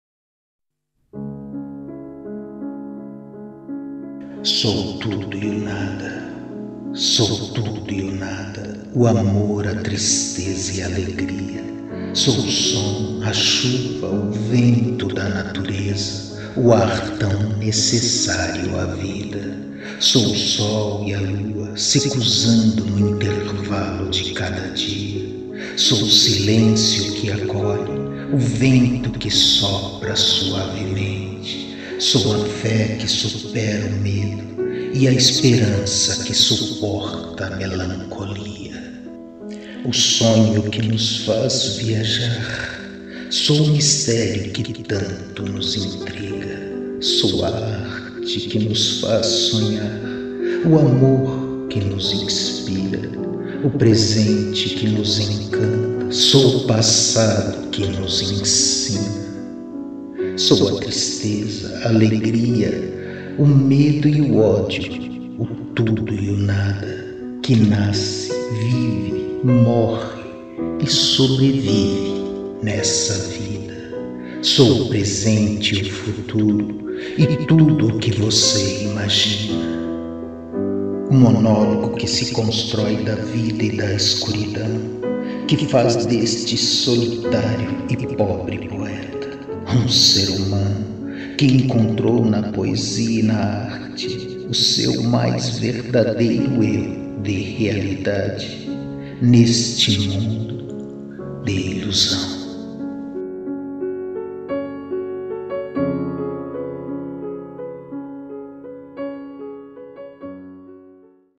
SOU O TUDO E O NADA _ EDUARDO BRODOWISKI _
SOU O PRESENTE E O FUTURO, E TUDO O QUE VOCÊ IMAGINA... O MONÓLOGO, QUE SE CONSTRÓI DA VIDA E DA ESCURIDÃO, QUE FAZ DESTE SOLITÁRIO E POBRE POETA, O ARTISTA, QUE SE ILUDIU COM A ARTE, E SE ALIENOU PARA AS COISAS DA VIDA!